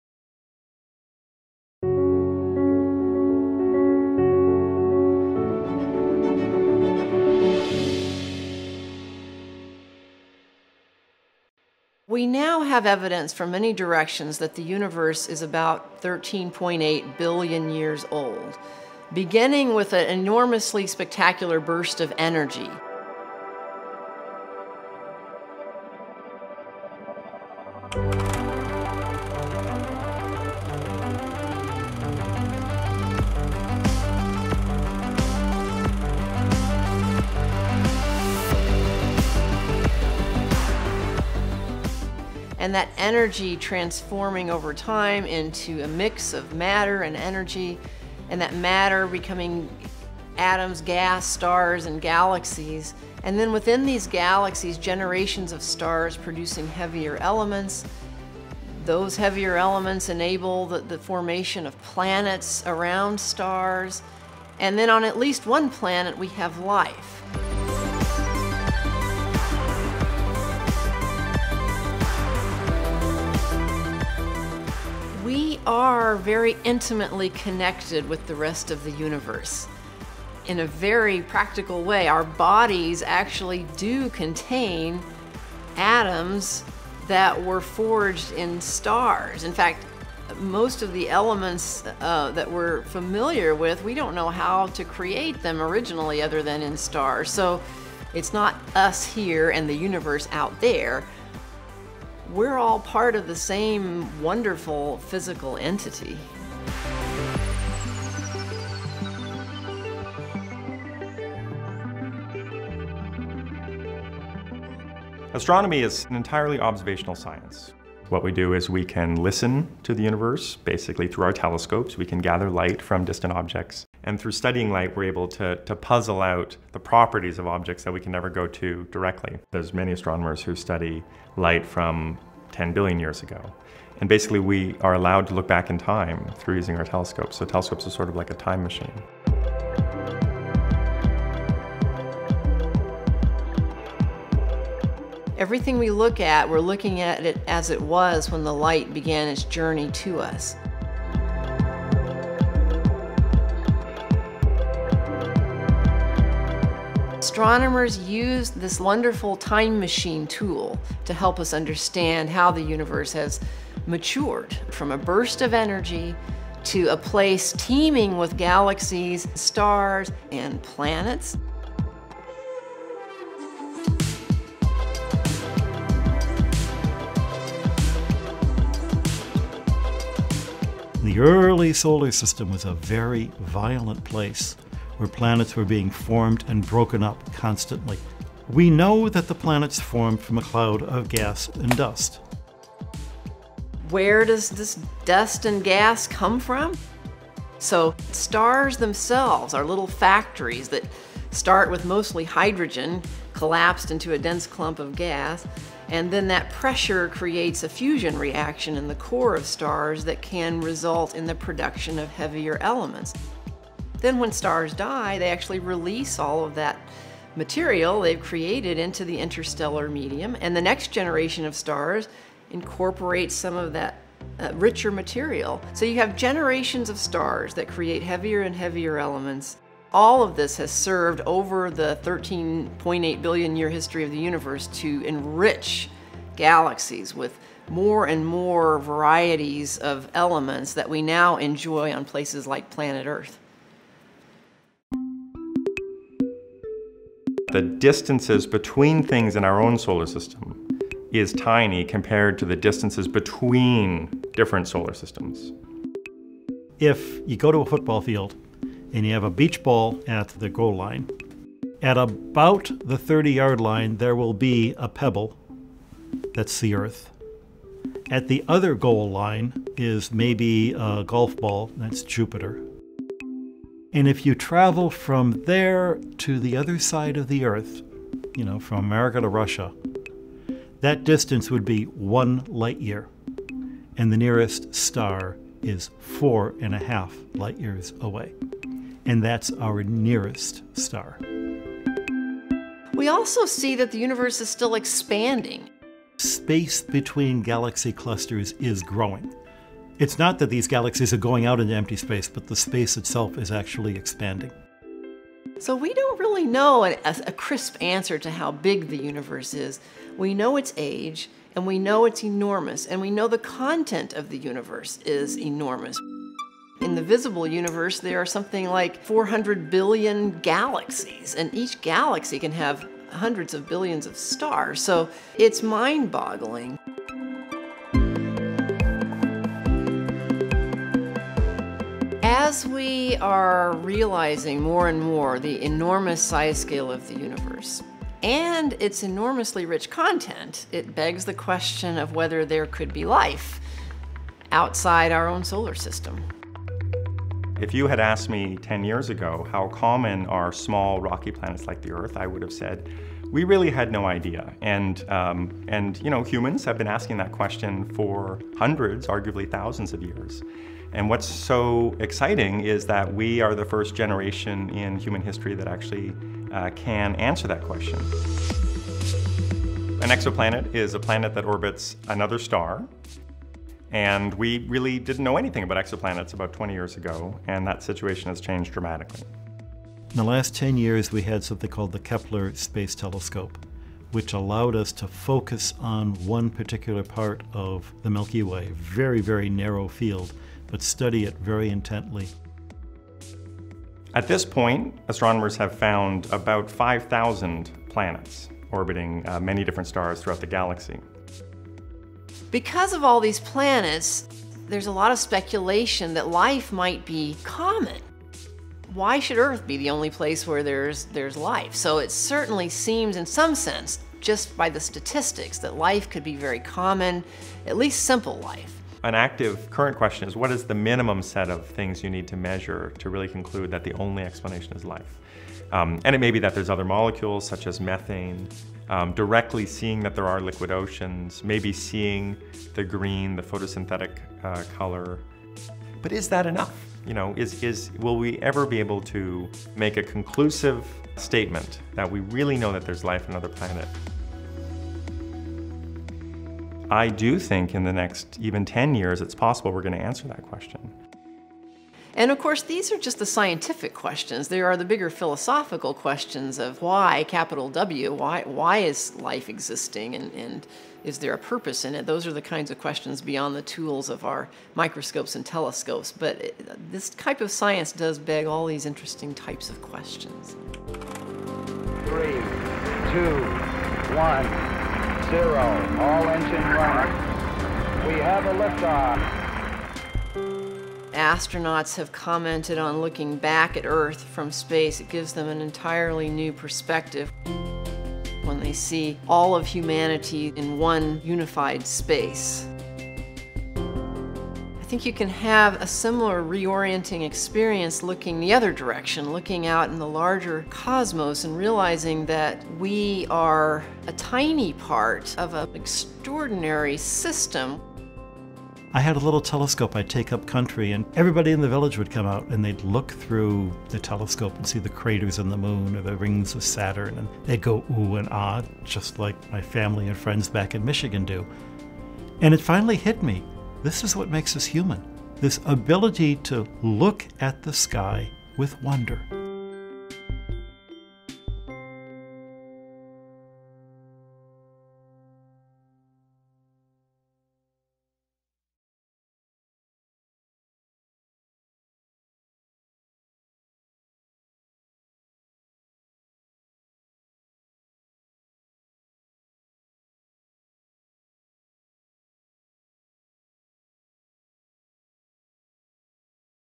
Humans in a Vast Universe: Astronomy and Cosmic Significance
How big is the universe? What is our place within it? As astronomers and astrophysicists study the vast expanse of the universe, they encounter new ideas about the origins of this expanse and the significance of humans within it. In this film, Drs. Guy Consolmagno, Jennifer Wiseman, and David Charbonneau explore humanity’s connection with the universe, the expanding nature of the universe, and the existence of exoplanets. They explain how looking outward at our galaxy and the estimated 400 billion galaxies around us should evoke a sense of awe and stimulate inquisitive minds to ask bigger philosophical questions.

This video was produced by the AAAS Dialogue on Science, Ethics, and Religion (DoSER) program as a part of its Science for Seminaries project.